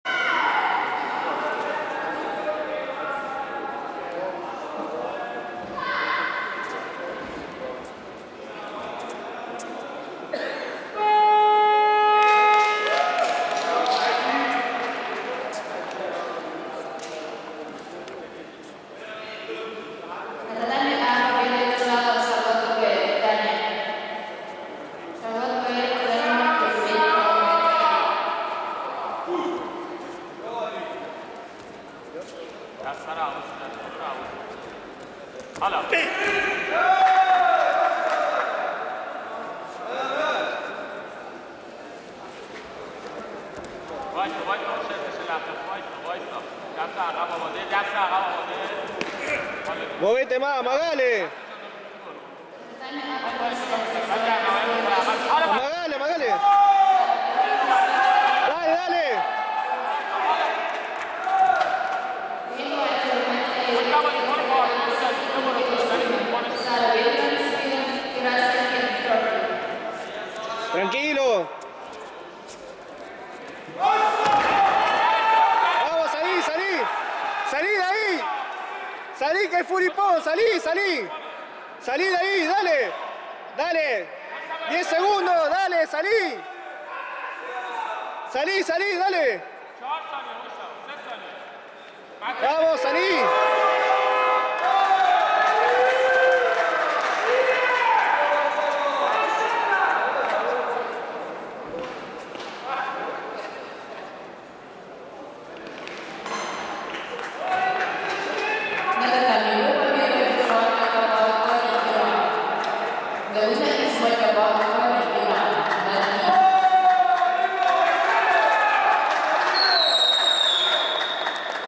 WCG - 25.10.2013 - AGUIRRE Ricardo Vs AKBARI Mojtaba (Iran)
El Sensei Ricardo es derrotado rápidamente por el Iraní